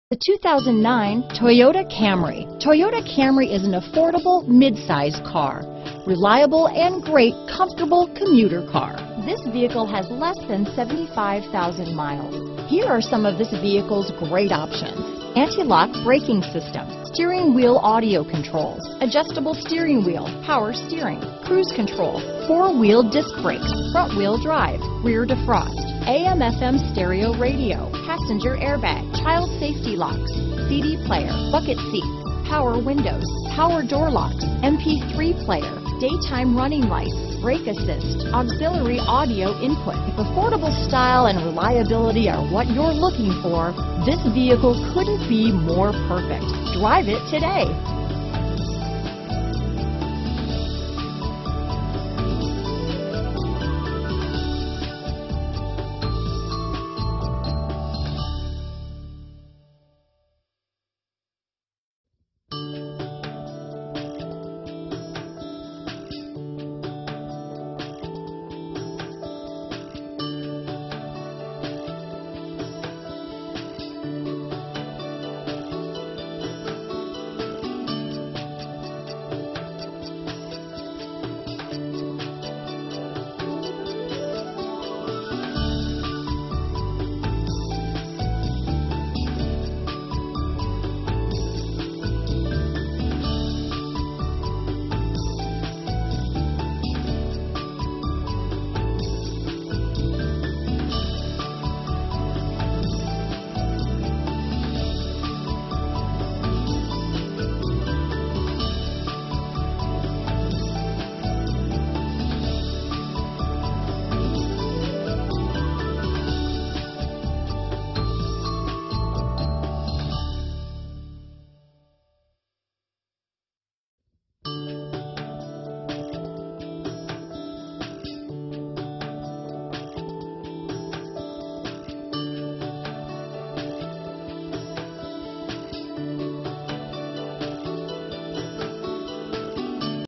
2009 Toyota Camry Wilson, Rocky Mount, Raleigh, Wake Forest, Zebulon, NC 12469A
2009 Toyota Camry

ONE OWNER, LOCAL TRADE, and 100% SERVICED AND FRONT LINE READY. Bisque w/Meteor Fabric Seat Trim. In a class by itself! Wow! Where do I start?! Medlin Buick GMC Mazda is excited to offer this gorgeous-looking 2009 Toyota Camry. A spacious car that gets great fuel mileage... Why torture yourself driving a small commuter box up and down the highway every day when you can ride in roomy comfort. New Car Test Drive said, ...boring and bland are out. Flash and dash are in... A Consumer Guide Recommended Buy for 2009 & 2008.